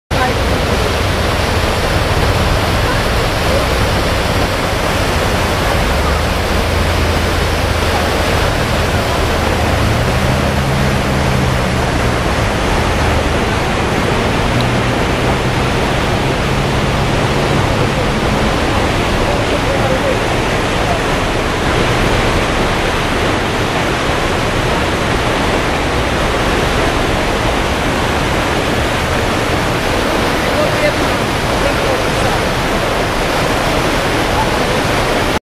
Curacao, Zuikertuin-2, Hoosbui na Tomas, 6 nov 2010
De hoosbui op 6 november 2010 maakt goed zichtbaar hoe kwetsbaar Curacao nog is na Tomas. Hier beelden van Zuikertuin Mall.